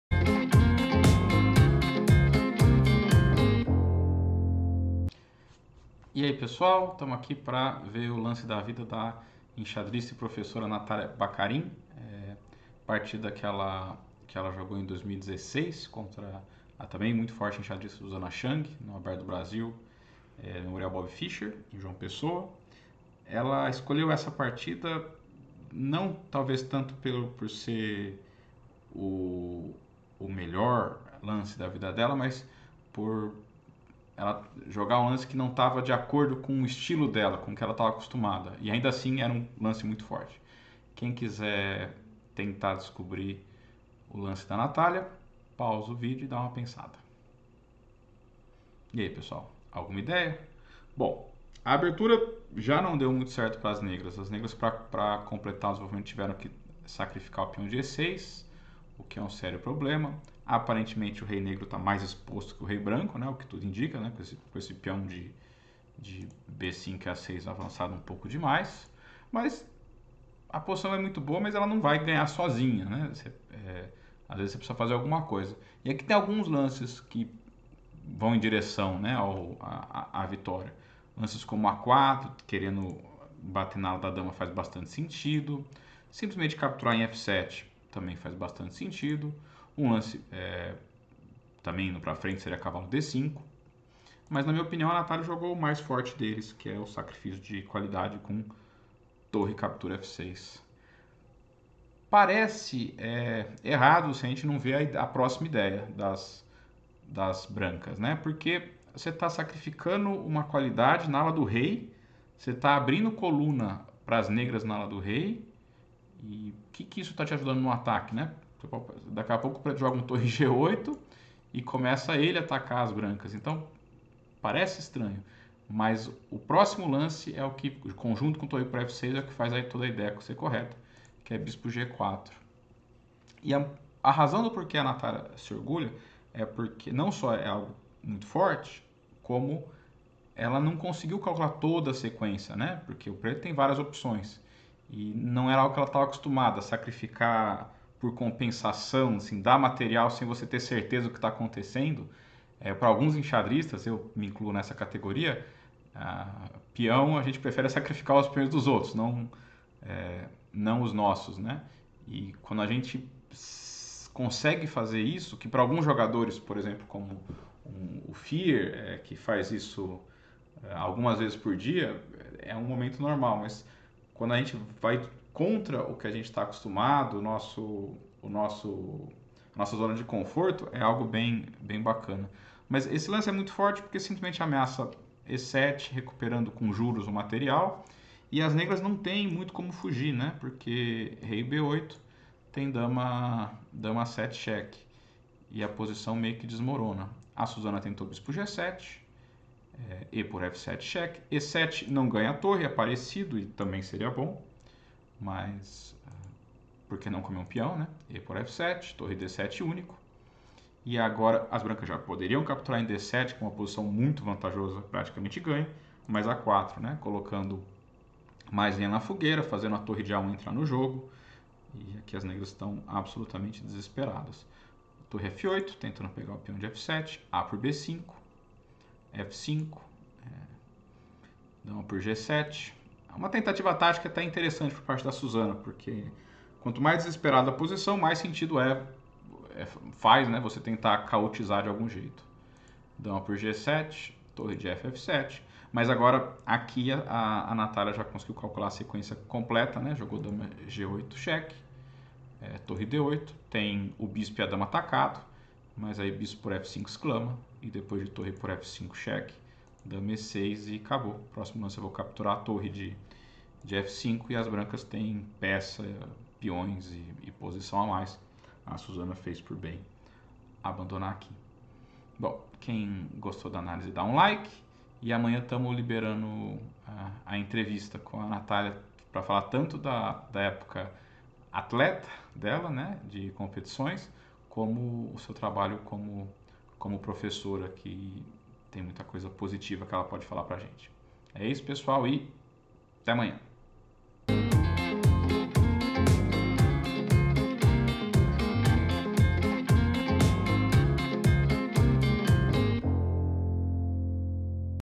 Lance da Vida: Natália Baccarin
Olá, pessoal!

A enxadrista e professora de xadrez Natália Baccarin escolheu como Lance da Vida um forte sacrifício que mistura cálculo com intuição para converter uma boa posição no ponto inteiro!

Fen: 2kr1b1r/1bq2ppp/p2pPn2/1p6/3QP3/2N1B3/PPP1B1PP/R4RK1 w - - 0 13

Bom proveito!
Felipe El Debs e Ana Vitória Rothebarth